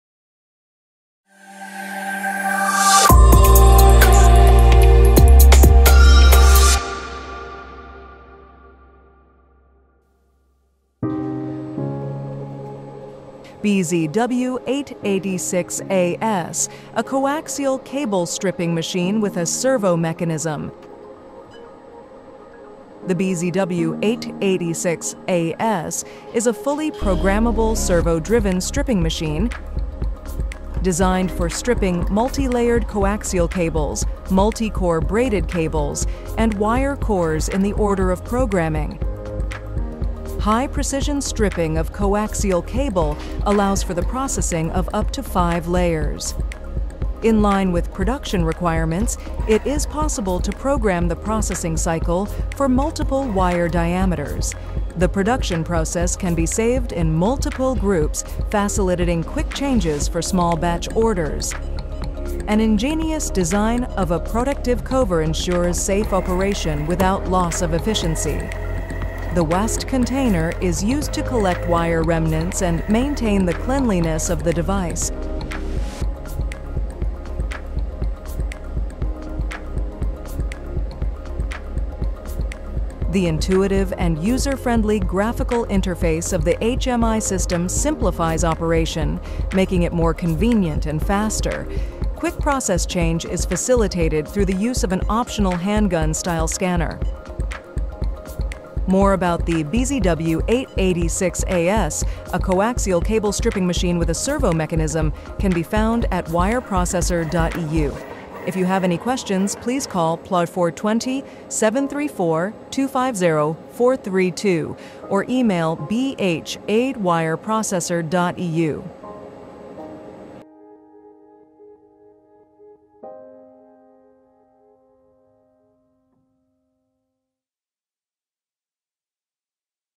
BZW-886AS - coaxial cable stripping machine with a servo mechanism BOZWANG
BZW-886AS a coaxial cable stripping machine with a servo mechanism

Technical Parameters :
BZW-886AS Servo coaxial stripping machine
Power Supply -80 - 264VAC 47 - 63Hz
Air source - 4 - 7bar
Power - 450W
Weight - 22KG
Overall dimension - L520 x W155 x H360mm
Screen size - 4.3”
Cutting range wire OD diameter ≤6.5mm
Max layers 5 layer
Cutting feed increment - 0.01mm
Length increment  - 0.1mm
Maximum cutting length - 40mm